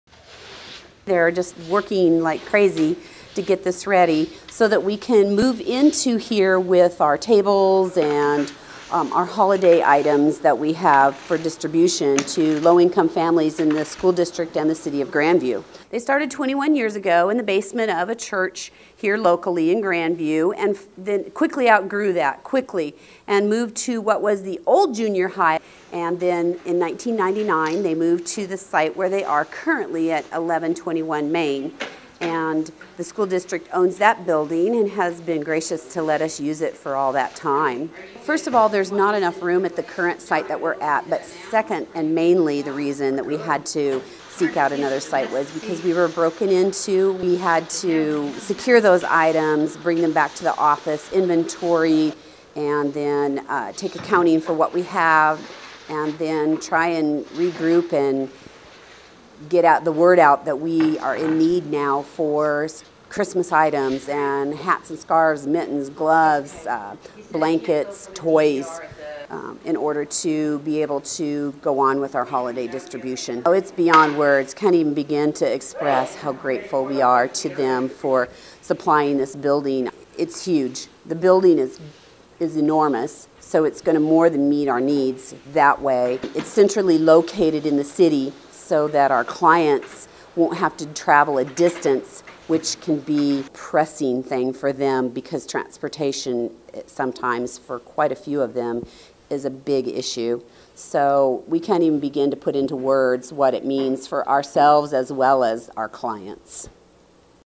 GC-4 Assists GAP on Clean-Up Day
Continuing to nurture a partnership more than 20 years old, the Grandview C-4 School District is letting the Grandview Assistance Program (GAP) use the former KinderCare building for their holiday supply distribution